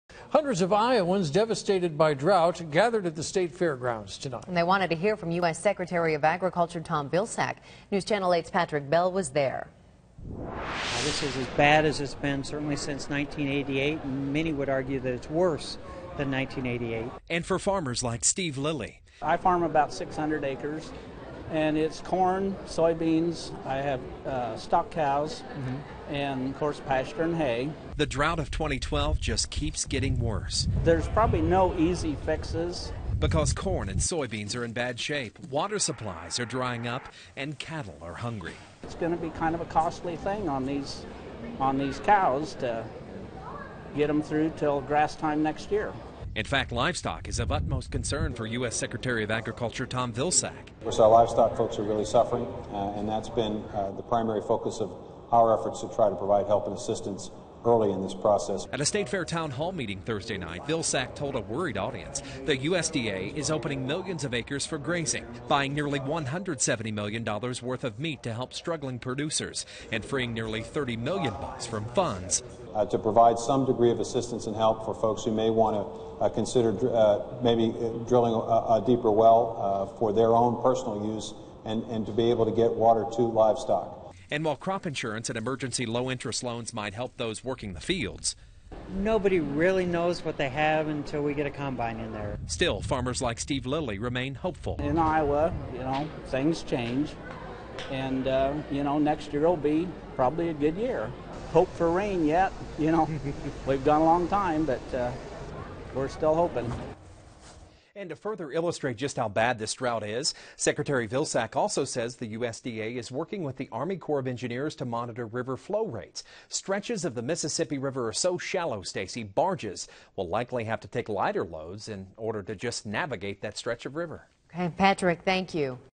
Drought meeting with Vilsack draws hundreds
Hundreds of Iowans devastated by drought gathered at the fairgrounds Thursday night to hear from U.S. Secretary of Agriculture Tom Vilsack.